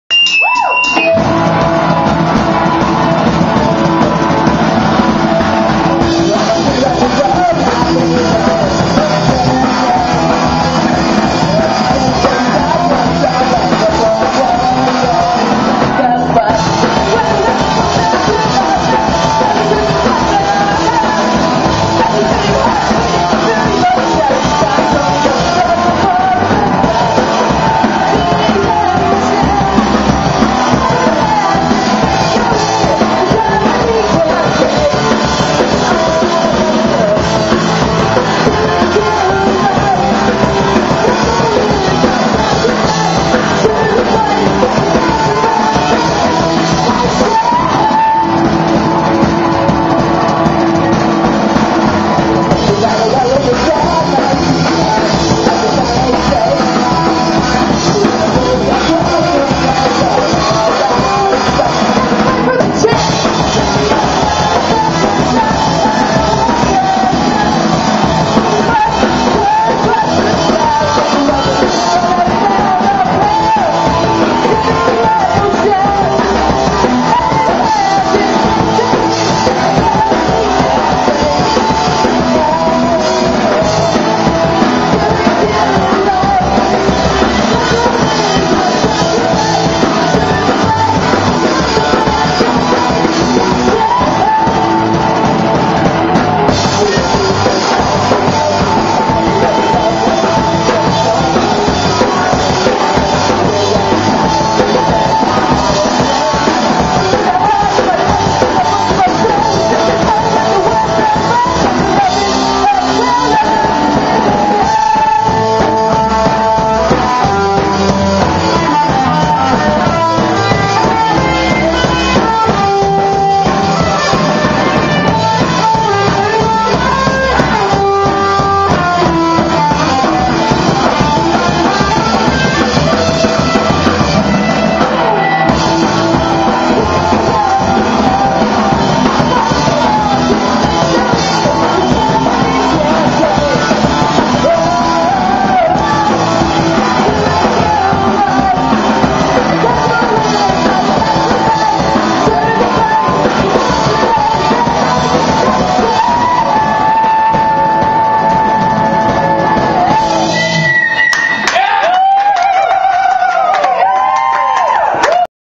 Innuendo @ the Doll Hut (7/29/10) #4
Innuendo live @ the world famous Doll Hut in Anaheim, California. Recorded on July 29, 2010.